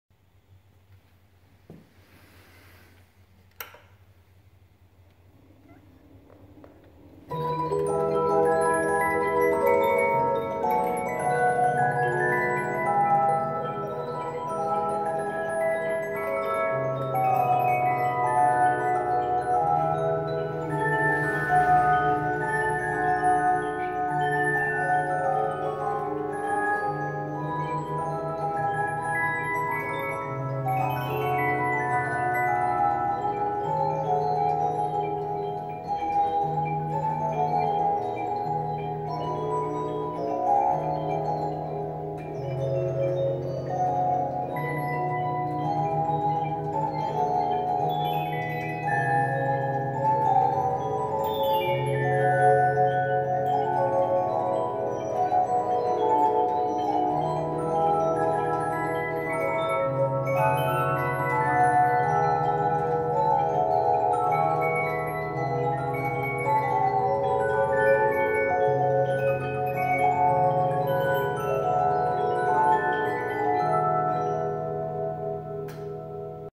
An imposing Swiss mandoline quatuor by Conchon with marquetry rosewood inlaid case, made circa 1870.
Listen to this very large and rare music box by Conchon of Geneva. The large diameter and very wide cylinder plays extremely diverse and long melodies. Both the case and the mechanism are of the best quality and execution. Gude & Meis exceptional clocks and music boxes Amsterdam.